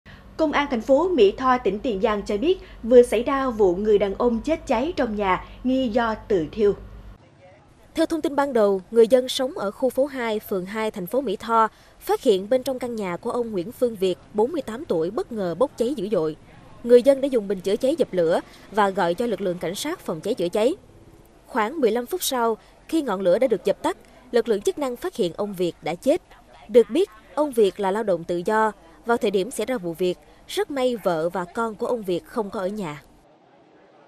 Người đàn ông 48 tuổi chết cháy trong nhà nghi do tự thiêu
Chiều 19-2, Công an tỉnh Tiền Giang đang điều tra một vụ người đàn ông chết cháy trong nhà trên địa bàn phường 2, TP. Mỹ Tho.